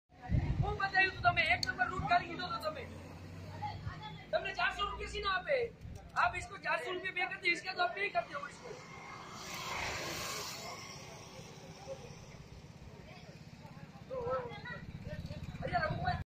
Getting Read in Jeep Safari For Gir National Park Gujarat India | Gautam and Gautam Group
Gir Forest National Park is a wildlife sanctuary in Gujarat, western India. It was established to protect Asiatic lions, who frequent the fenced-off Devalia Safari Park, along with leopards and antelopes. Gir Jungle Trail, outside the fenced area, traverses deciduous forest and is home to wildlife including vultures and pythons. The Kamleshwar Dam has marsh crocodiles and birds, such as Indian skimmers and pelicans